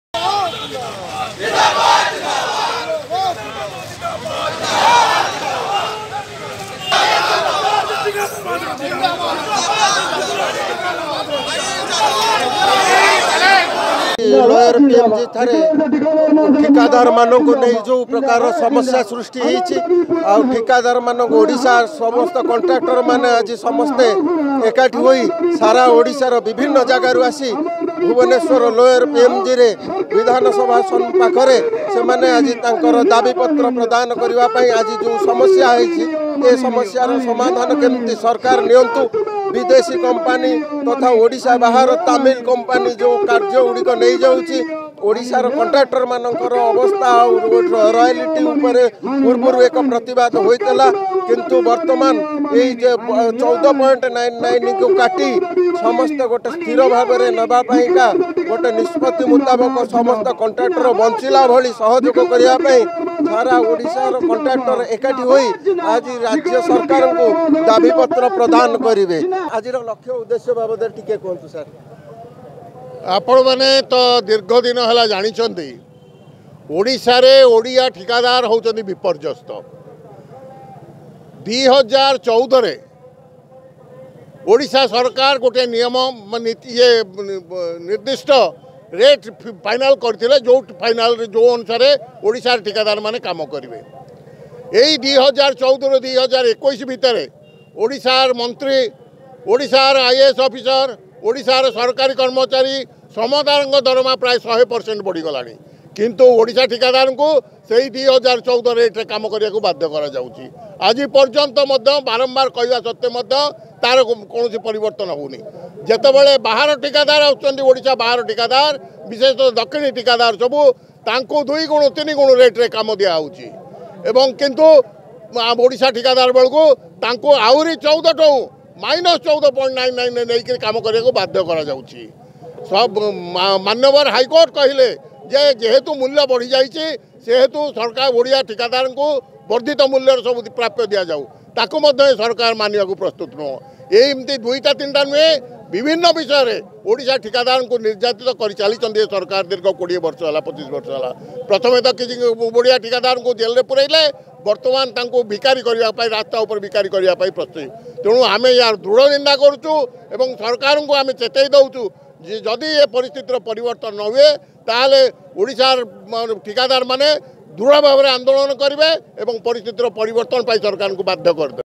ଭୁବନେଶ୍ୱର : ଓଡ଼ିଶାର ଠିକାଦାର ମହାସଂଘ ତରଫରୁ ଲୋୟର ପିଏମଜି ଠାରେ ବିରାଟ ଶୋଭାଯାତ୍ରା
ଓଡ଼ିଶାର ଠିକାଦାର ମହାସଂଘ ତରଫରୁ ଲୋୟର ପିଏମଜି ଠାରେ ବିରାଟ ଶୋଭାଯାତ୍ରା

[ODIA ] Official YouTube Channel Of Sadhuta News Odisha . An Initiative By Sadhuta Network To Connect Citizens To The Latest Information About Odisha .